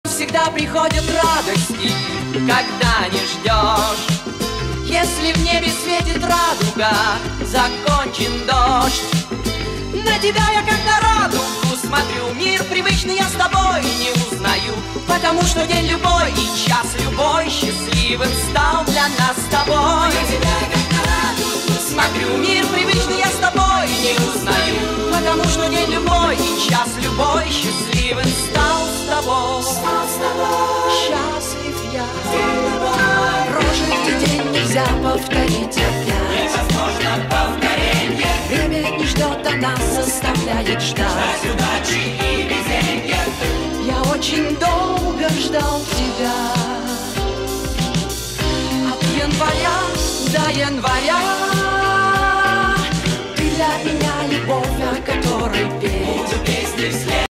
22 апреля над Ромодановом весь вечер висела коромыслом радуга - к счастью!
Верите ли вы в приметы? Говорят, что радуга приносит счастье! Как вы думаете, правда ли это? Пишите комментарии...